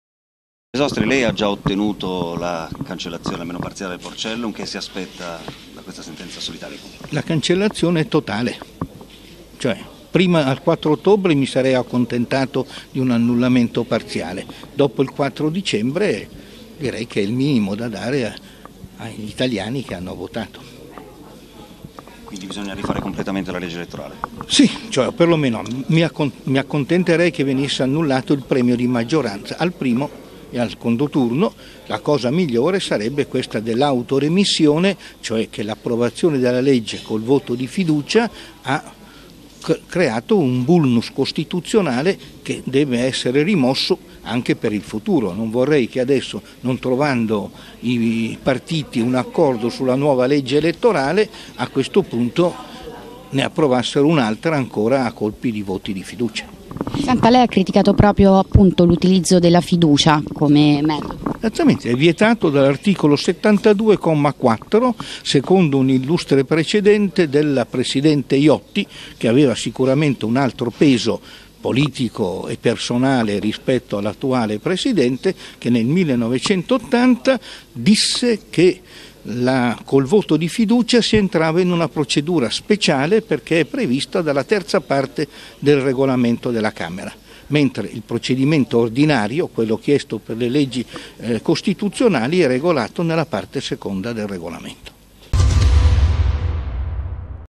CONSULTA, AVV BESOSTRI, CANCELLAZIONE IL MINIMO
“Mi aspetto la cancellazione totale dell’Italicum. Mi pare il minimo per gli italiani dopo il voto del 4 dicembre. Mi aspetto sia annullato il premio di maggioranza al primo e al secondo turno.